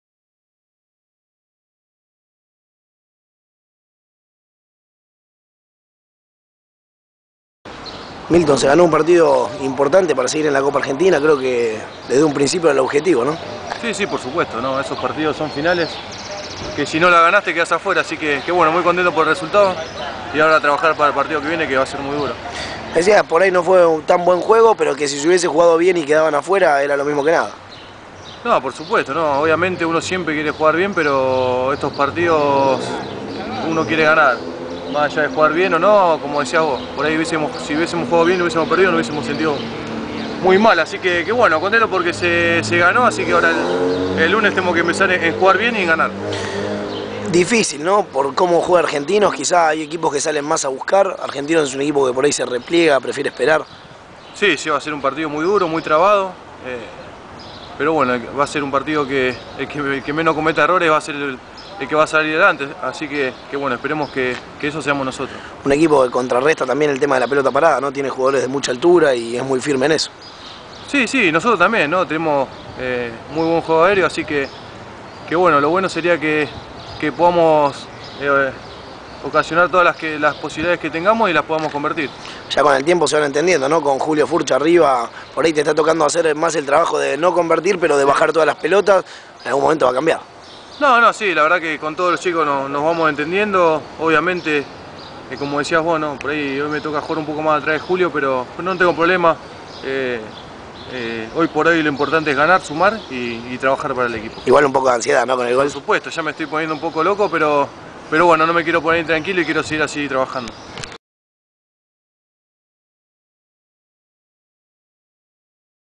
Caraglio: "No me tengo que poner intranquilo y debo seguir trabajando para el gol"
Milton Caraglio se afianza con Julio Furch como la dupla delantera, aunque aún no pudo llegar al gol confía en que llegará pronto.